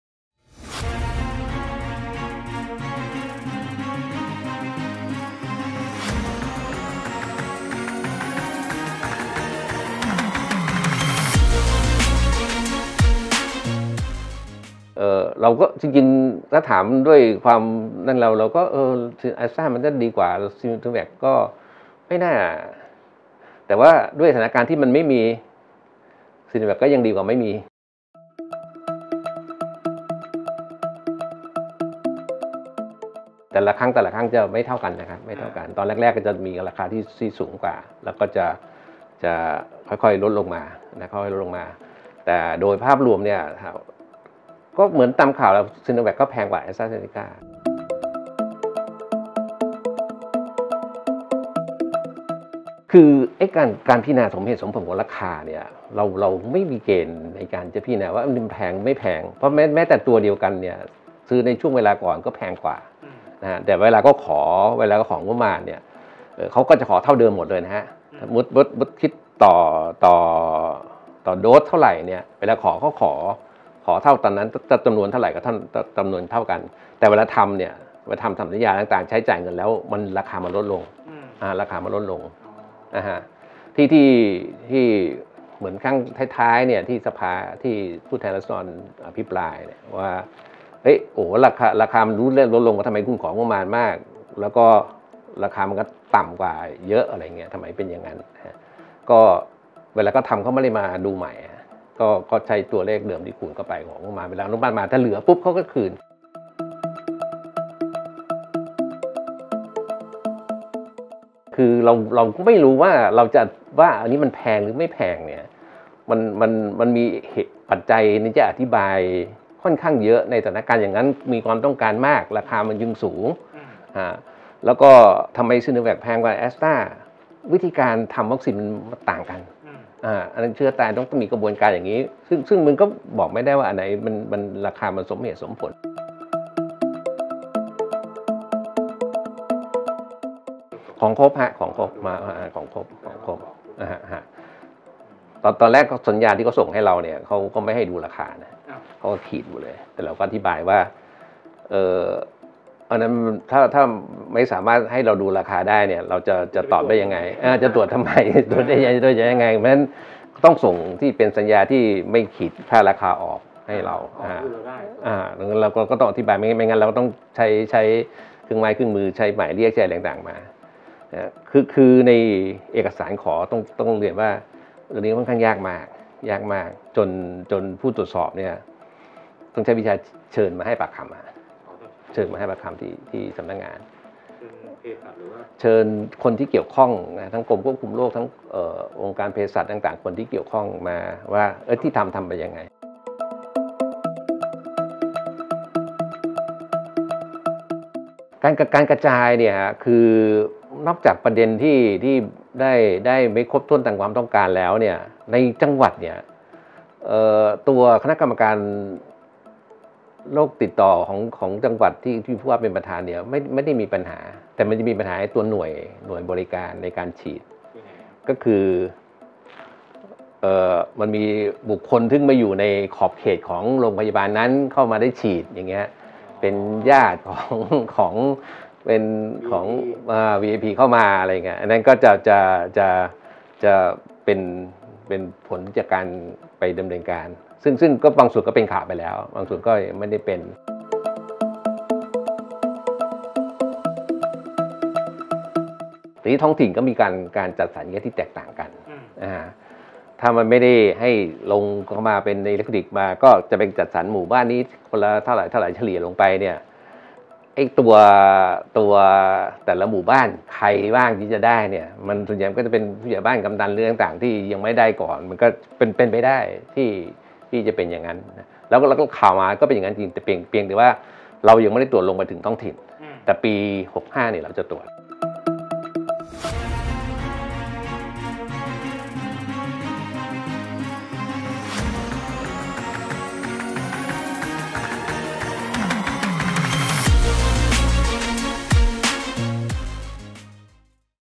ล้วงผลสอบ สตง.ชำแหละ รบ.บิ๊กตู่ จัดซื้อวัคซีนโควิด 1.7 หมื่นล. l สำนักข่าวอิศรา
อ่านเพิ่มเติม : ISRA -WHY?  ล้วงผลสอบ สตง.ชำแหละ รบ.บิ๊กตู่ จัดซื้อวัคซีนโควิด 1.7 หมื่นล.

กด Subscribe-กดกระดิ่ง  เพื่อติดตามคลิปใหม่ๆ จากสำนักข่าวอิศราอย่างรวดเร็ว